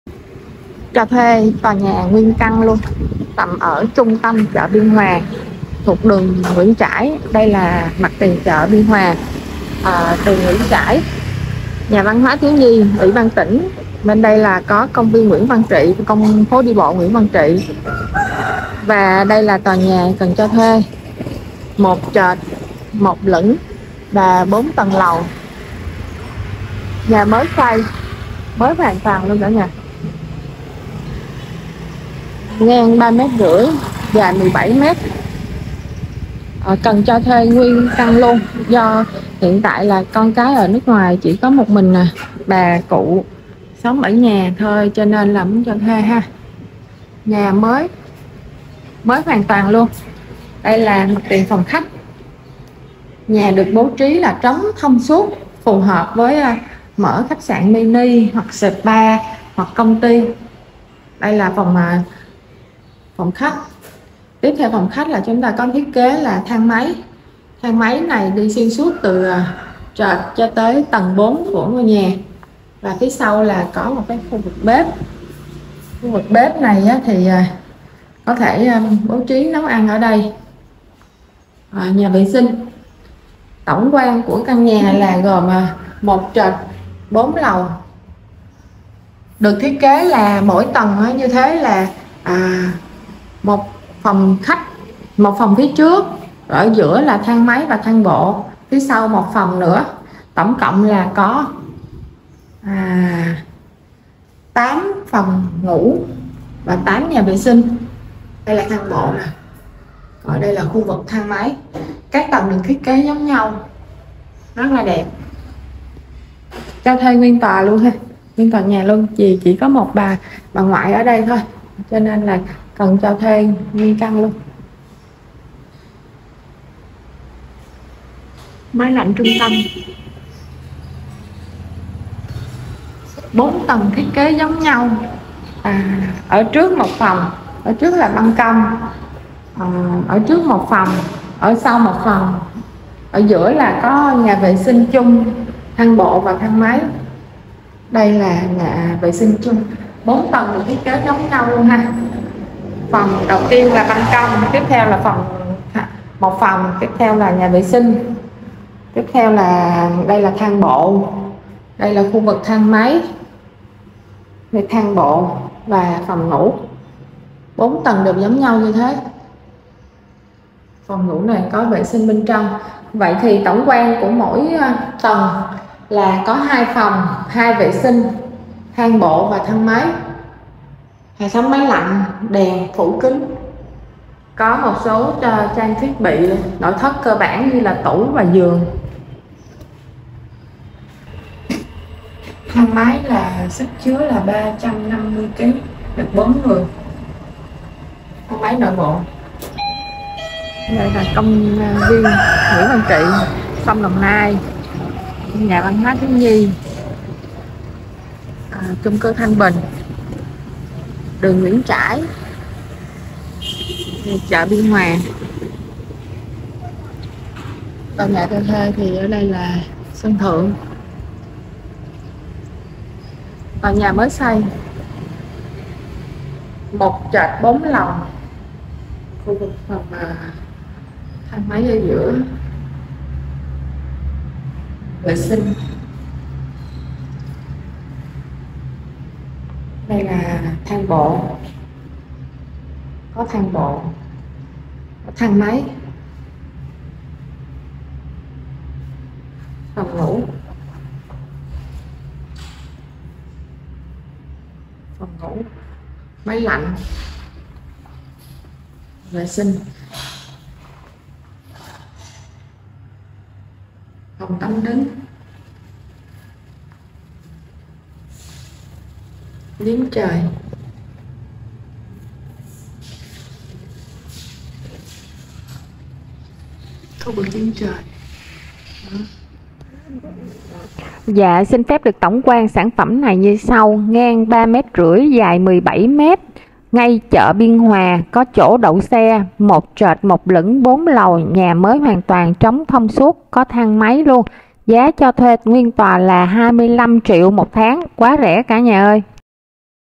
Cho thuê nguyên căn toà nhà mini , ngay chợ Biên Hoà tp Biên Hoà tỉnh Đồng nai phù hợp mở spa…
✅Cần cho thuê toà nhà mini! 1 trệt 1 lửng, 4 lầu, có thang máy!
✅ Địa chỉ: mặt tiền chợ Biên Hoà tp Biên Hoà tỉnh Đồng Nai
✅ Diện tích: 3,5 x 17 có thang máy + thang bộ
✅Có đủ nội thất, nhà mới xây , thiết kế đẹp và hiện đại, gần trục đường chính có thể đậu xe, ngay chợ biên hoà, xung quanh có ngân hàng, siêu thị, cơ quan nhà nước như uỷ ban tỉnh, chung cư Thanh Bình, chi cục thuế…
✅Phù hợp mở spa, văn phòng công ty, trung tâm anh ngữ, khách sạn mini, văn phòng…
✅Giá rẻ: chỉ 25 triệu tháng